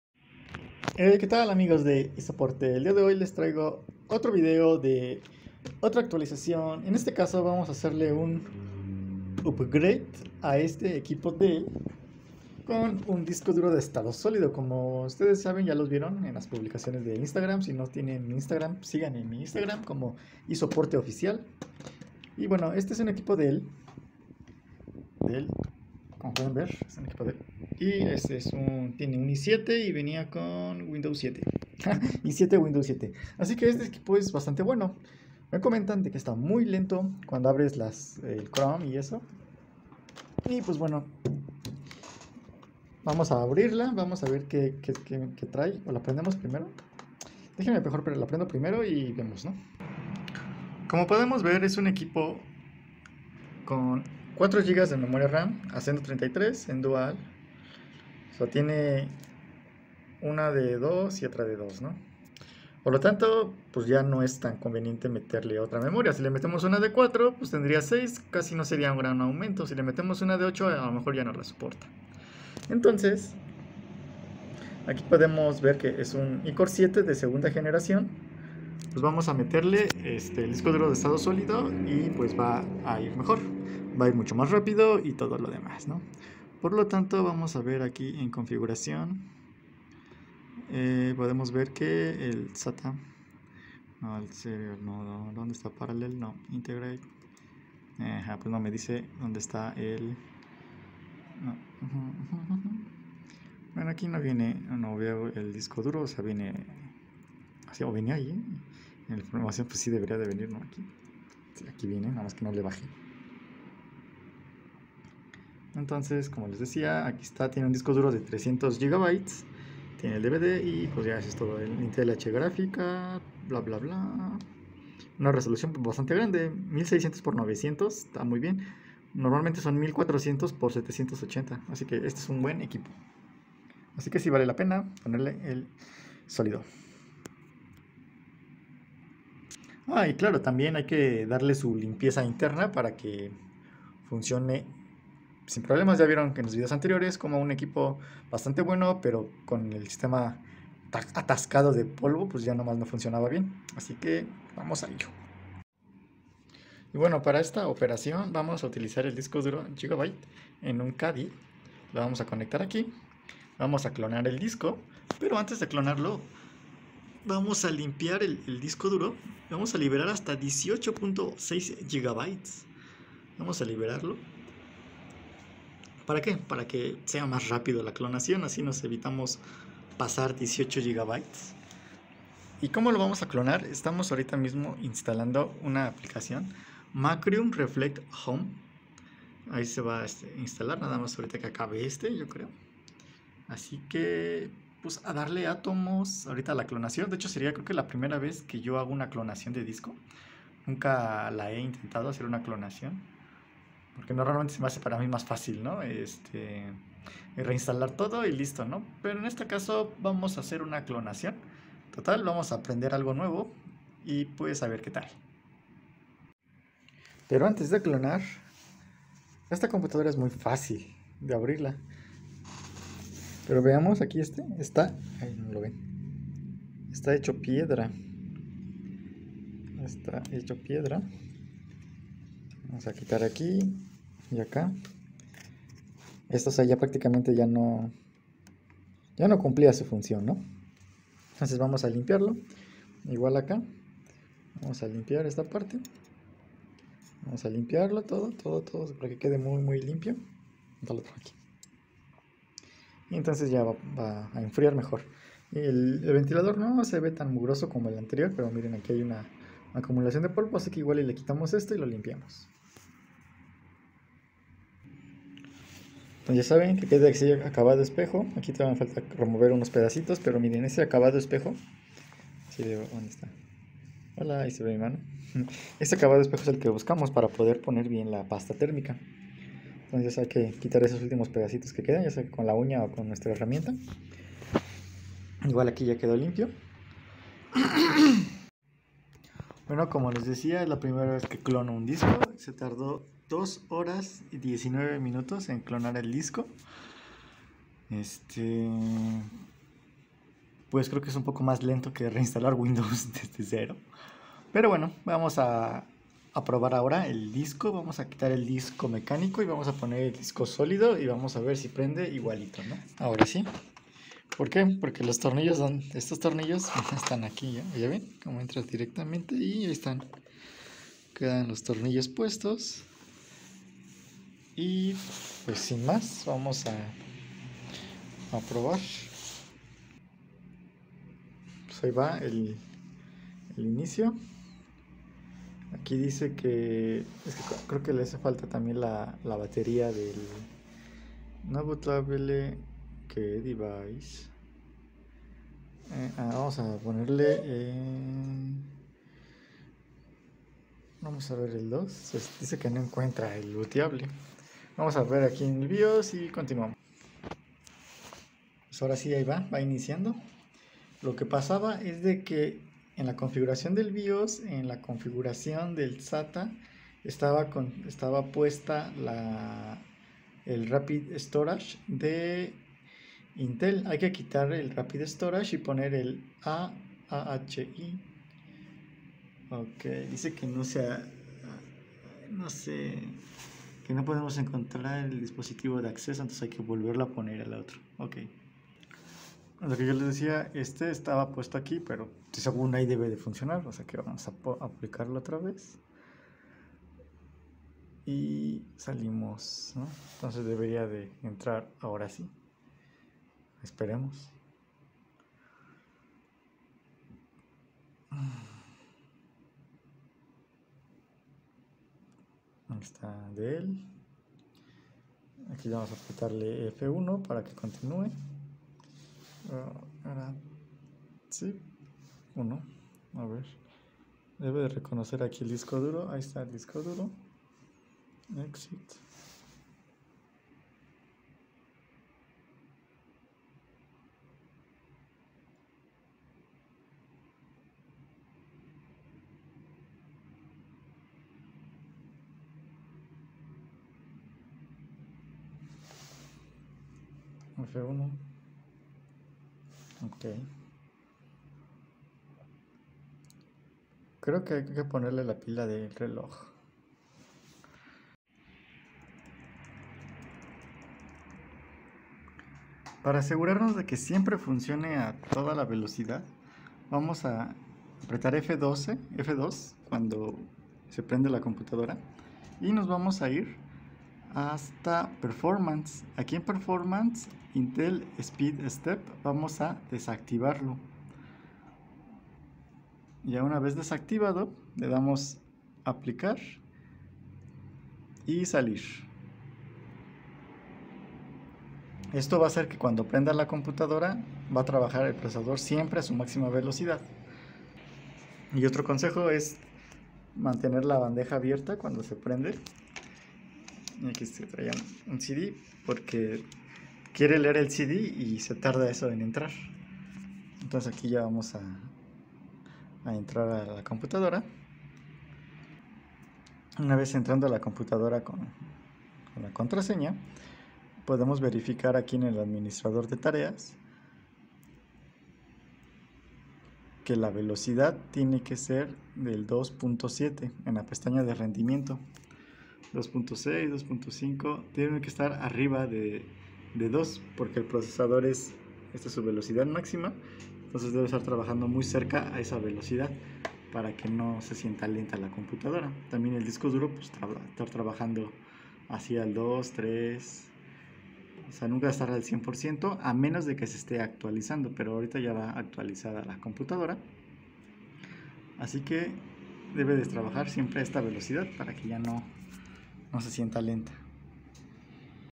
Dell Latitude E6520 Mejorar velocidad
Te explico como mejor tu equipo con un SSD y limpieza básica